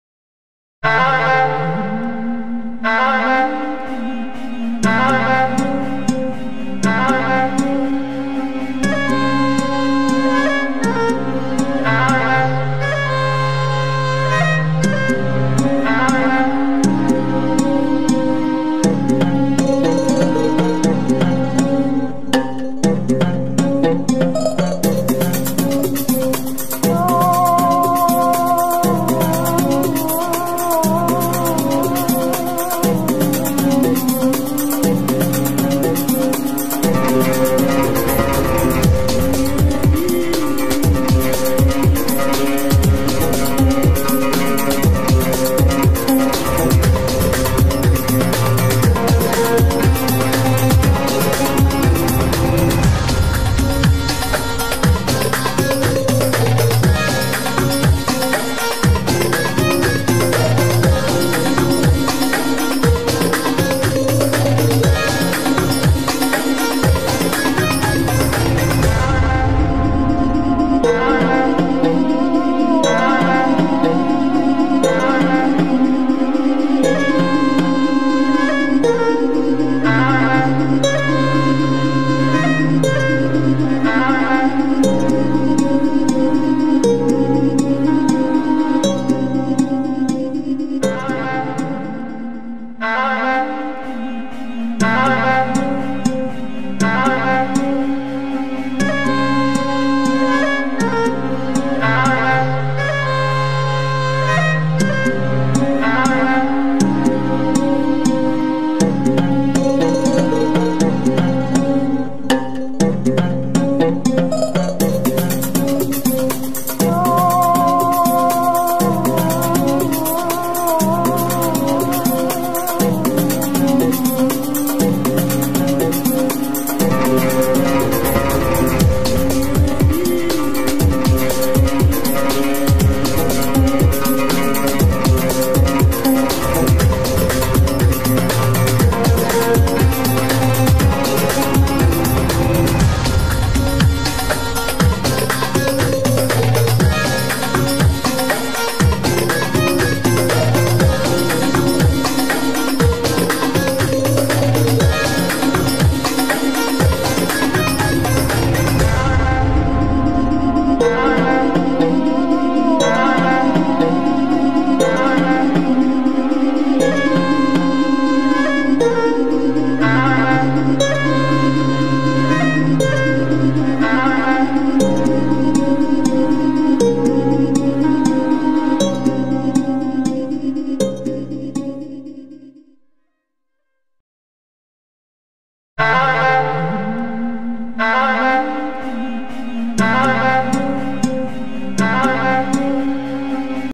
hard working Spider MeHandi Design
Assalam o alaikoom it's really beautifull mehandi design the Labour spider make her home because home sweet home